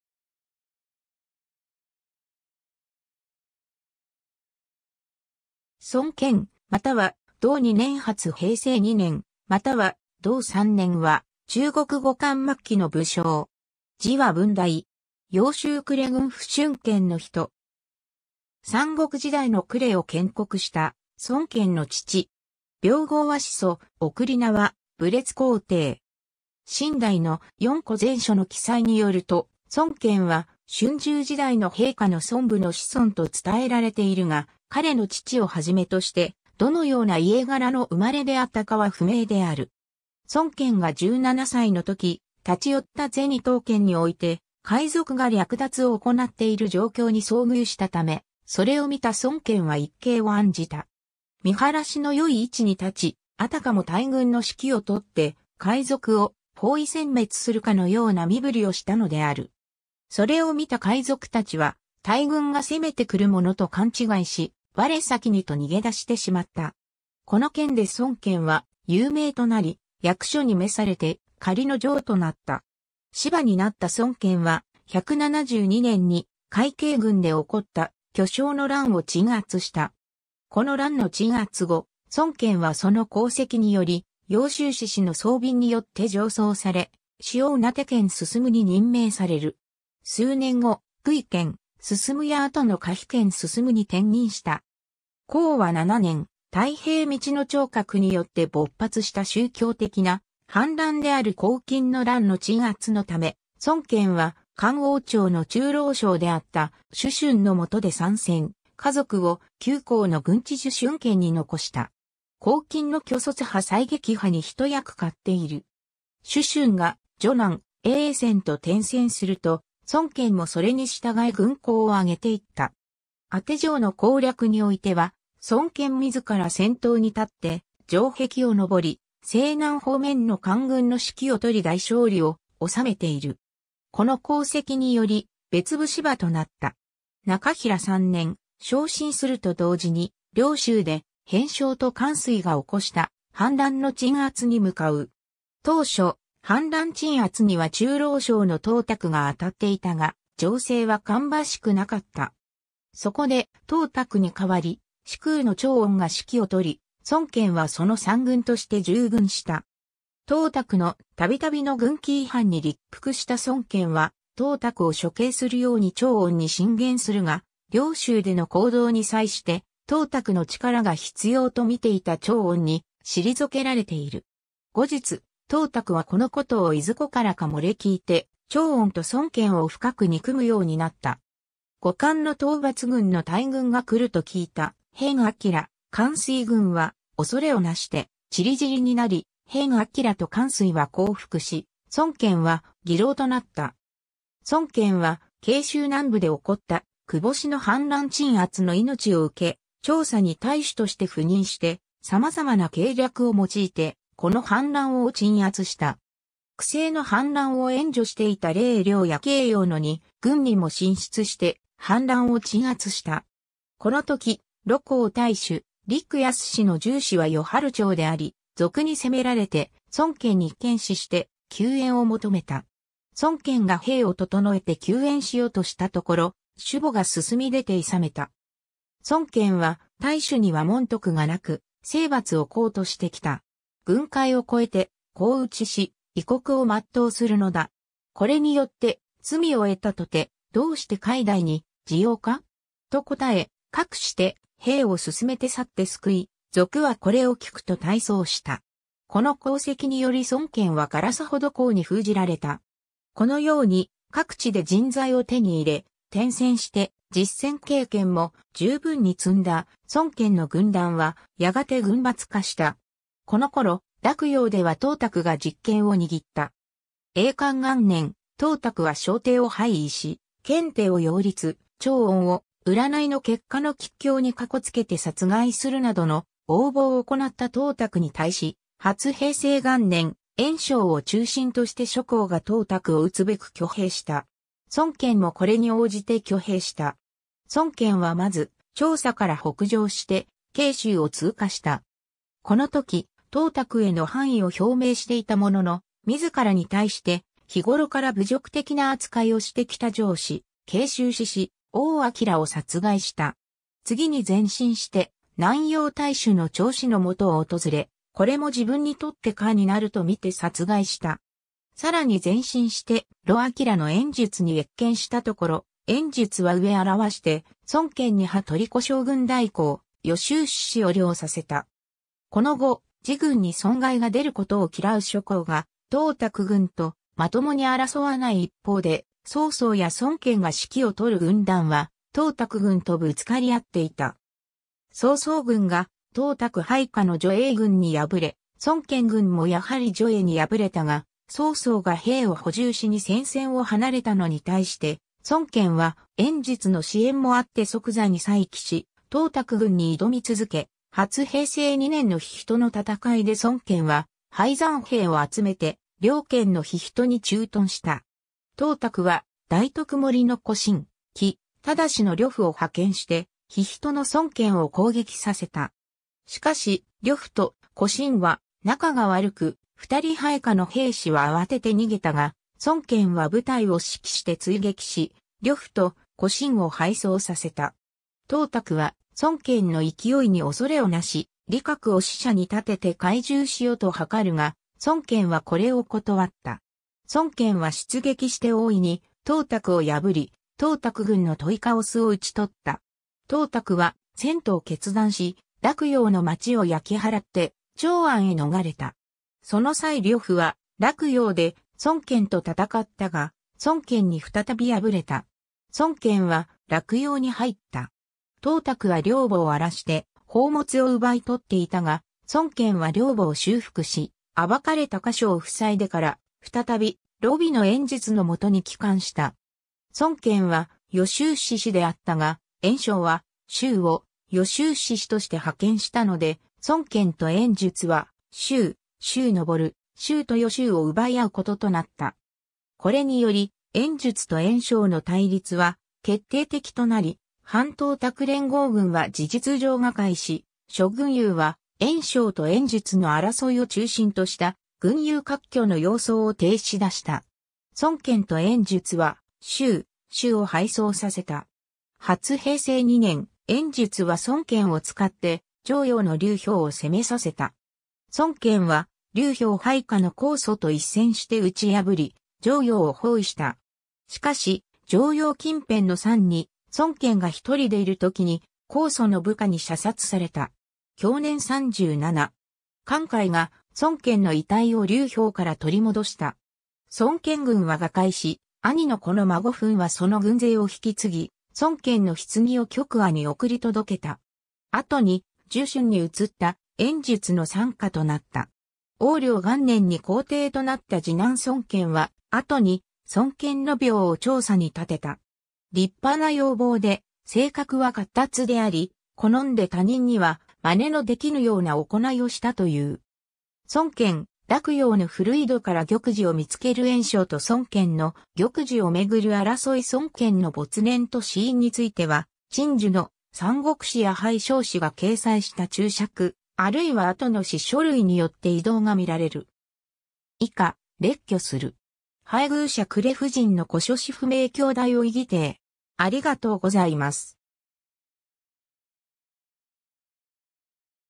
孫堅
孫 堅（そん けん、永寿元年（155年）または同2年（156年） - 初平2年（191年）または同3年（192年））は、中国後漢末期の武将。
字は文台。
揚州呉郡富春県の人。
三国時代の呉を建国した孫権の父。
廟号は始祖、諡は武烈皇帝。
 清代の『四庫全書』の記載によると、孫堅は春秋時代の兵家の孫武の子孫と伝えられているが、彼の父を初めとして、どのような家柄の生まれであったかは不明である。
 孫堅が17歳の時、立ち寄った銭唐県において、海賊が略奪を行なっている状況に遭遇したため、それを見た孫堅は一計を案じた。
見晴らしの良い位置に立ち、あたかも大軍の指揮を執って、海賊を包囲殲滅するかのような身振りをしたのである。
それを見た海賊たちは、大軍が攻めてくるものと勘違いし、我先にと逃げ出してしまった。
この件で孫堅は有名となり、役所に召されて仮の尉（警察・軍事担当）となった。
 司馬（軍事を司る職）になった孫堅は、172年に会稽郡で起こった許昌の乱を鎮圧した。
この乱の鎮圧後、孫堅はその功績により揚州刺史の臧旻によって上奏され、塩瀆県丞に任命される。
数年後、盱眙県丞や後の下邳県丞に転任した。
 光和7年（184年）、太平道の張角によって勃発した宗教的な反乱である黄巾の乱の鎮圧のため、孫堅は漢王朝の中郎将であった朱儁の下で参戦、家族を九江の郡治寿春県に残した。
黄巾の渠帥波才撃破に一役買っている。
朱儁が汝南・潁川と転戦すると、孫堅もそれに従い軍功をあげていった。
宛城の攻略においては、孫堅自ら先頭に立って城壁を登り、西南方面の官軍の指揮を執り大勝利を収めている。
この功績により、別部司馬となった。
 中平3年（186年）、昇進すると同時に、涼州で辺章と韓遂が起こした反乱の鎮圧に向かう。
当初、反乱鎮圧には中郎将の董卓があたっていたが、情勢は芳しくなかった。
そこで董卓に代わり、司空の張温(後漢書董卓伝では、朝廷復以司空張溫為車騎將軍，假節…としている)が指揮を執り、孫堅はその参軍として従軍した。
董卓の度々の軍規違反に立腹した孫堅は、董卓を処刑するように張温に進言するが、涼州での行動に際して董卓の力が必要と見ていた張温に退けられている。
後日、董卓はこの事をいずこからか漏れ聞いて、張温と孫堅を深く憎むようになった。
 後漢の討伐軍の大軍が来ると聞いた辺章・韓遂軍は、恐れをなして散りぢりになり、辺章と韓遂は降伏し、孫堅は議郎となった。
 孫堅は荊州南部で起こった区星の反乱鎮圧の命を受け、長沙に太守として赴任して、様々な計略を用いて、この反乱を鎮圧した。
区星の反乱を援助していた零陵や桂陽の二郡にも進出して、反乱を鎮圧した。
この時、廬江太守陸康の従子は宜春長であり、賊に攻められて孫堅に遣使して救援を求めた。
孫堅が兵を整えて救援しようとしたところ、主簿が進みでて諫めた。
孫堅は 「太守には文徳が無く、征伐を功としてきた。郡界を越えて攻討し、異国を全うするのだ。これによって罪を獲たとて、どうして海内に媿（は）じようか?」と答え、かくして兵を進めて往って救い、賊はこれを聞くと退走した。
この功績により孫堅は烏程侯に封じられた。
 このように、各地で人材を手に入れ、転戦して実戦経験も十分に積んだ孫堅の軍団は、やがて軍閥化した。
 この頃、洛陽では董卓が実権を握った。
永漢元年（189年）、董卓は少帝を廃位し、献帝を擁立、張温を占いの結果の吉凶にかこつけて殺害するなどの横暴を行った董卓に対し、初平元年（190年）、袁紹を中心として諸侯が董卓を討つべく挙兵した。
孫堅もこれに応じて挙兵した。
孫堅はまず、長沙から北上して荊州を通過した。
この時、董卓への反意を表明していたものの、自らに対して日ごろから侮辱的な扱いをしてきた上司、荊州刺史王叡を殺害した。
 次に前進して南陽太守の張咨の元を訪れ、これも自分にとって禍になるとみて殺害した。
さらに前進して魯陽の袁術に謁見したところ、袁術は上表して孫堅に破虜将軍代行、豫州刺史を領させた。
 この後、自軍に損害が出ることを嫌う諸侯が董卓軍とまともに争わない一方で、曹操や孫堅が指揮を執る軍団は董卓軍とぶつかりあっていた。
曹操軍が董卓配下の徐栄軍に敗れ、孫堅軍もやはり徐栄に敗れたが、曹操が兵を補充しに戦線を離れたのに対して、孫堅は袁術の支援もあって即座に再起し董卓軍に挑み続け、初平2年（191年）の陽人の戦いで孫堅は敗残兵を集...